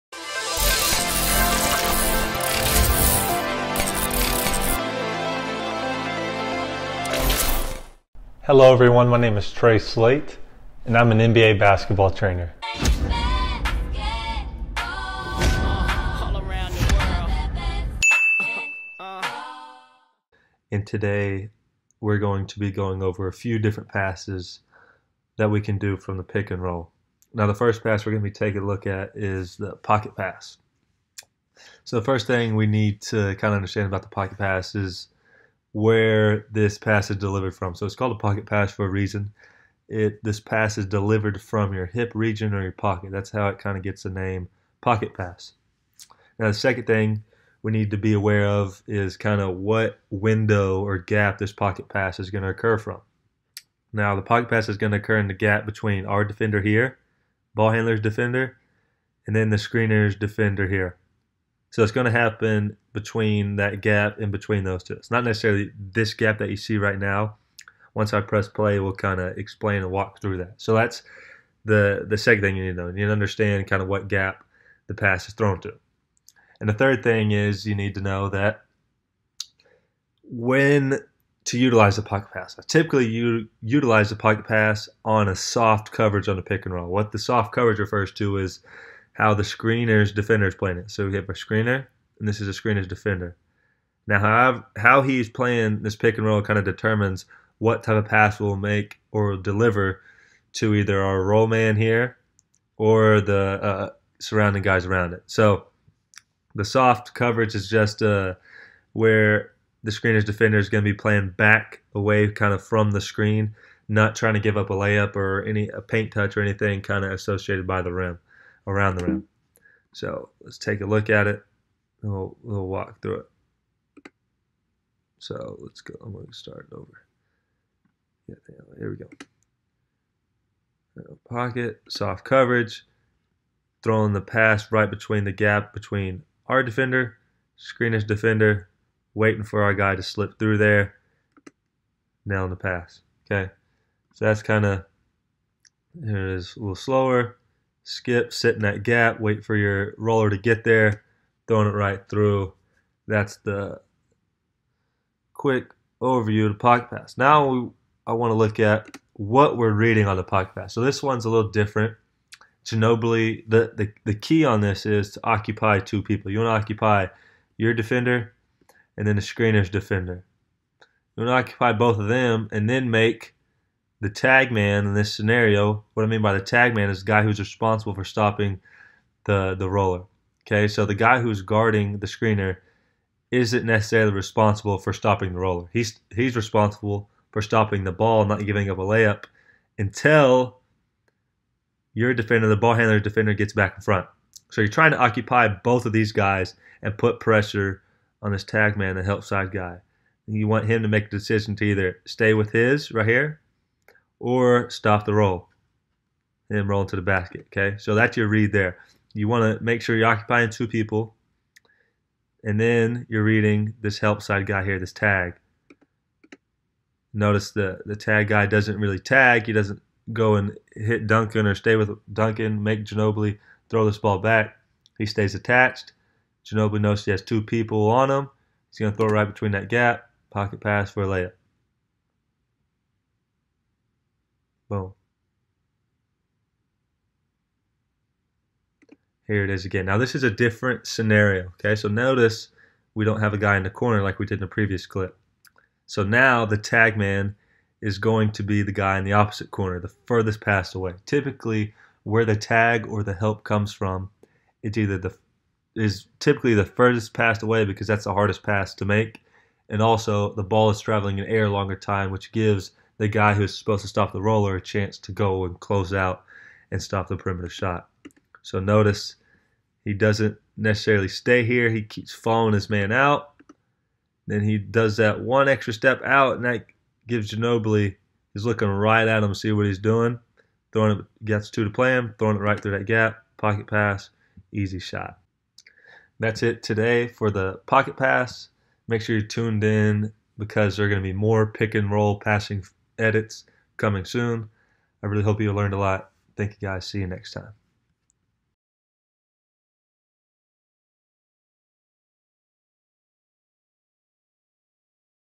NBA Film Edit: The Pocket Pass
NBA Trainer Trey Slate does an in-depth breakdown of the pocket pass. Coach Trey covers how to read the defense and when to perform the pocket pass.